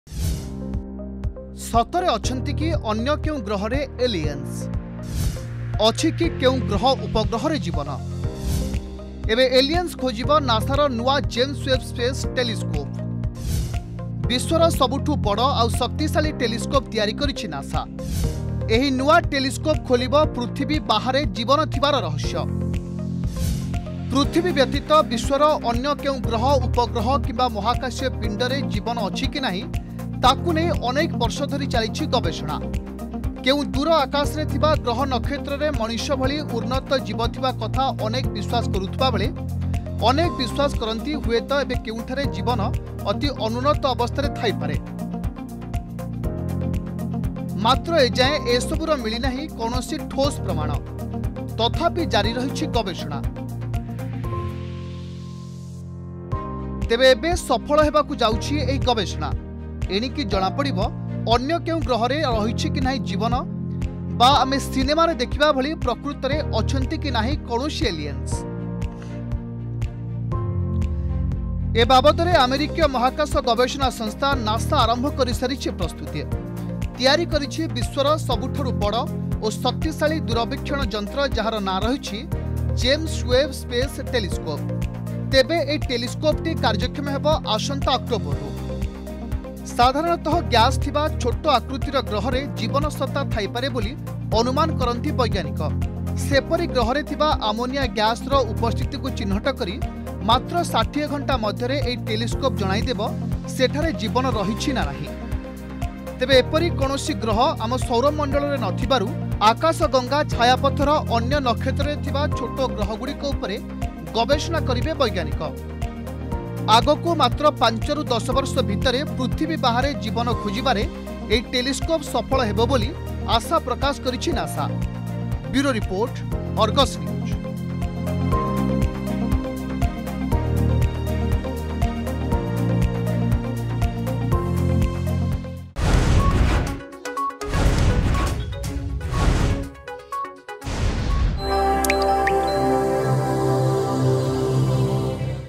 ଏଲିଏନ୍‌ସ ଖୋଜିବ ନାସାର ନୂଆ ଟେଲିସ୍କୋପ୍
ପୃଥିବୀ ବ୍ୟତୀତ ବିଶ୍ୱର ଅନ୍ୟ କେଉଁ ଗ୍ରହ ଉପଗ୍ରହ କିମ୍ବା ମହାକାଶୀୟ ପିଣ୍ଡରେ ଜୀବନ ଅଛି କି ନାହିଁ ତାକୁ ନେଇ ଅନେକ ବର୍ଷ ଧରି ଚାଲିଛି ଗବେଷଣା । ତେବେ ଏବେ ସଫଳ ହେବାକୁ ଯାଉଛି ଏହି ଗବେଷଣା । ଏଣିକି ଜଣା ପଡ଼ିବ ଅନ୍ୟ କେଉଁ ଗ୍ରହରେ ରହିଛି କି ନାହିଁ ଜୀବନ ବା ଆମେ ସିନେମାରେ ଦେଖିବା ଭଳି ପ୍ରକୃତରେ ଅଛନ୍ତି କି ନାହିଁ କୌଣସି ଏଲିଏନ୍‌ସ.